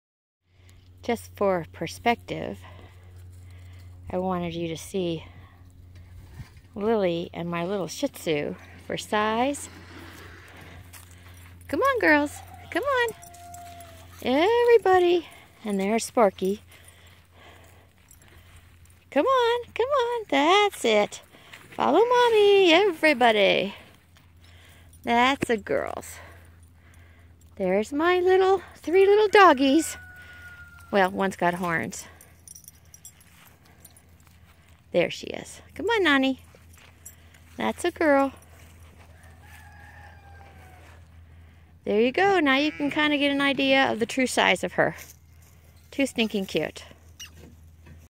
Mini Angora Goat
Gives you a good idea of the the size of this mini Angora. She is 6 months old and the newest addition to my fiber family!  Her name is Lily Squash Blossom!